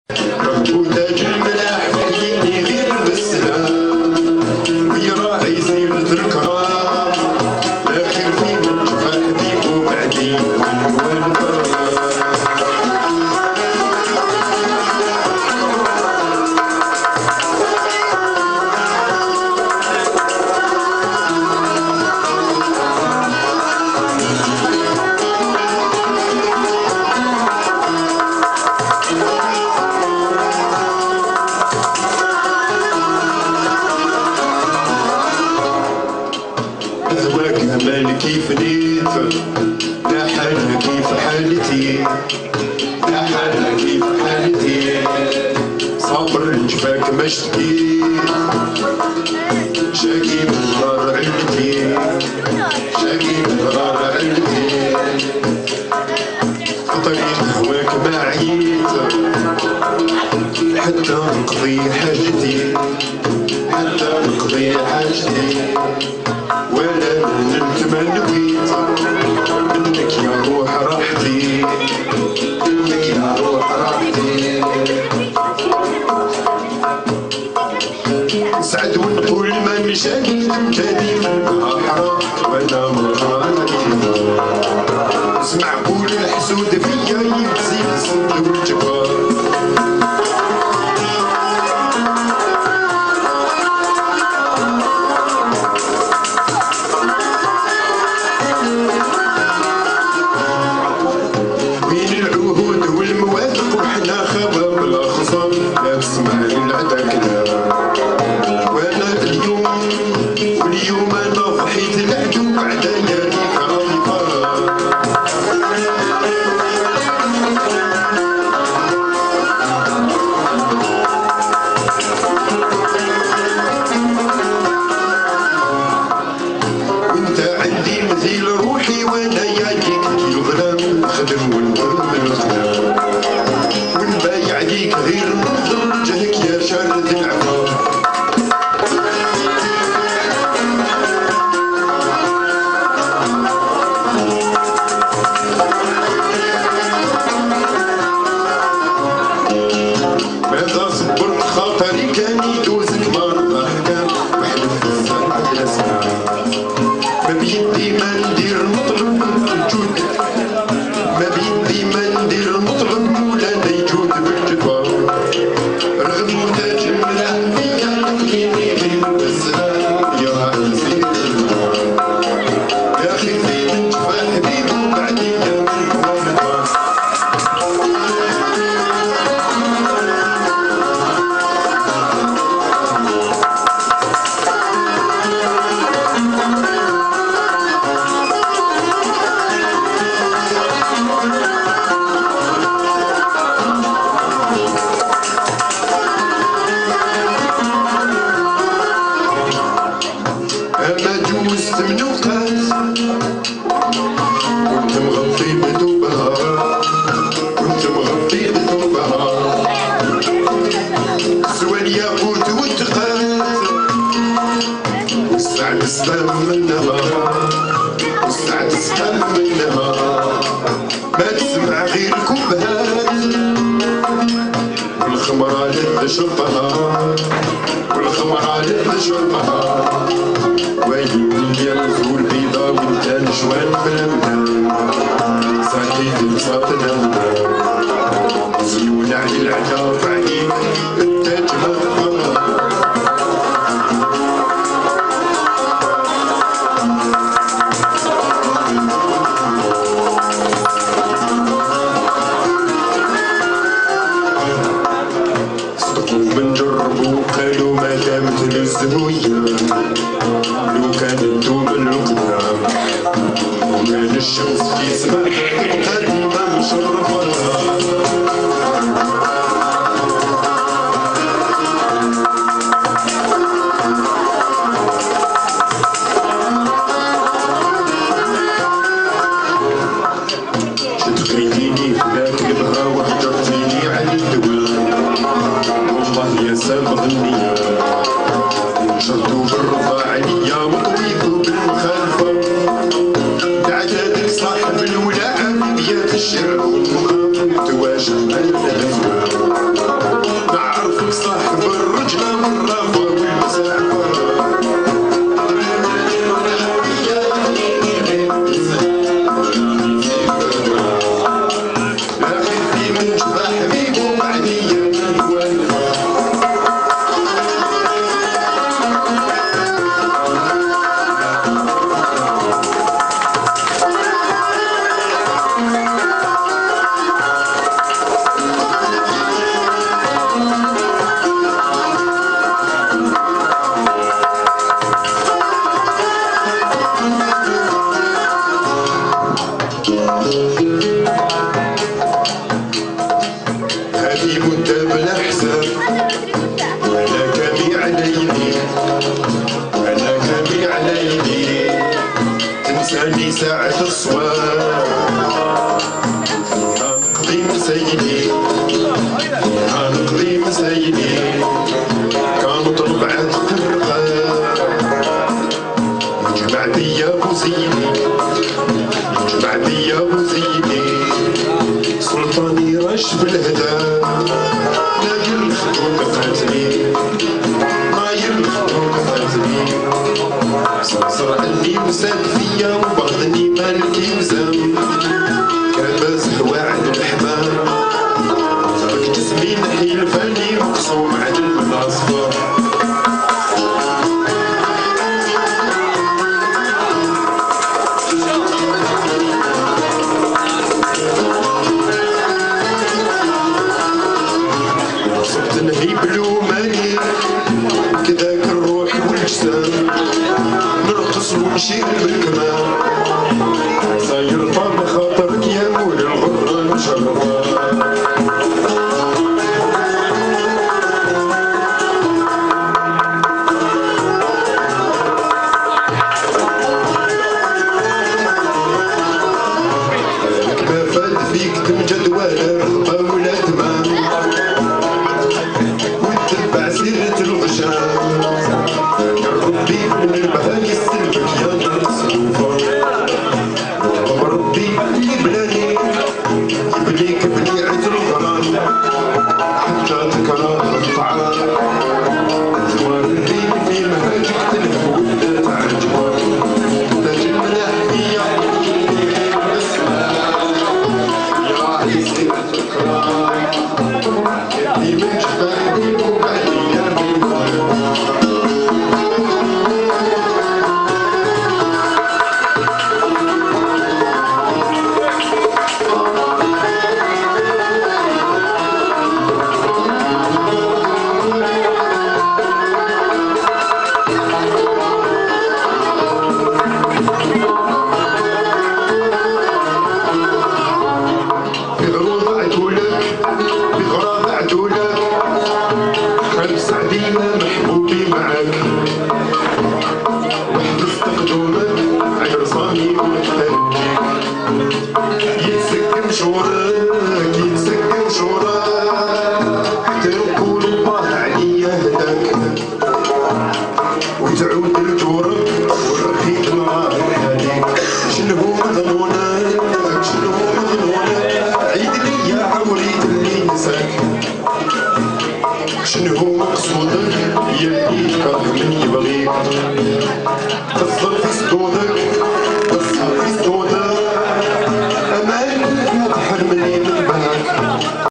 islam chabni chez la famille belaidi a cherchell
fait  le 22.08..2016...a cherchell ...
Tar;fethi hamza
f'hal ; boutka belkacem.
banjo ténore ; sofiane berouane.
mandole; chéikh islam chabni.
banjo guetare ; 3ami mustapha tainsa.
violent ; abdelatif saidji.
derbouka ; mezghrani med saghir.
photos ; mehdi keddache.
védio filmé part ; 3ami med cherchelli.
sonorisation ; nabil chamaine.